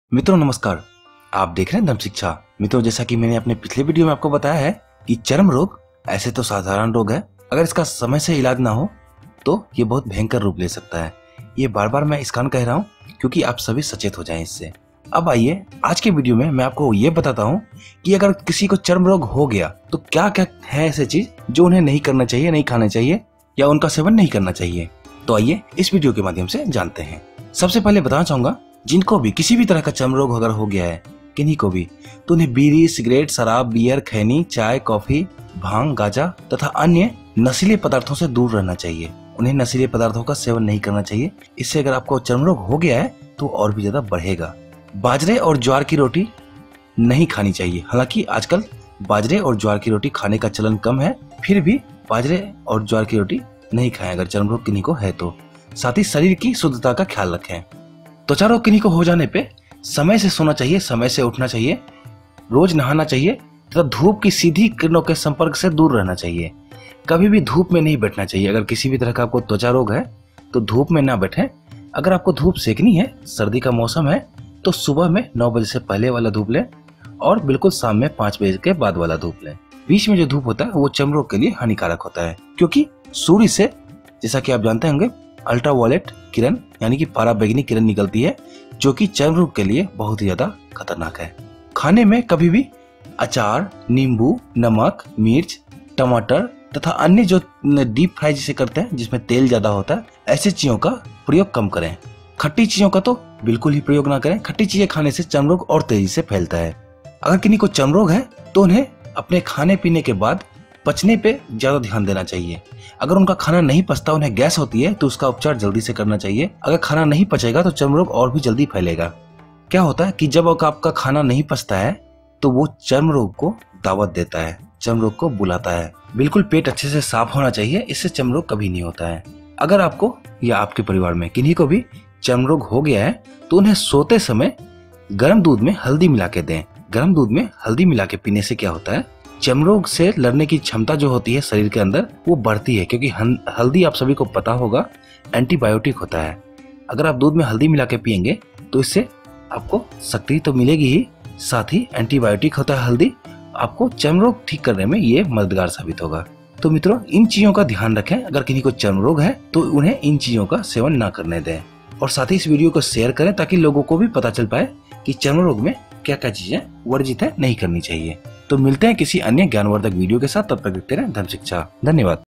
चर्मरोग हो जाने पे ये चीजें बिलकुल ना खाएं
चर्म रोग मुख्यतः खाने-पीने में अनियमितता से होता है। इसमें दवाई के अलावा परहेज का महत्वपूर्ण योगदान होता है। विडियो देख के आप जान पाएंगे कि क्या खाना है क्या नहीं।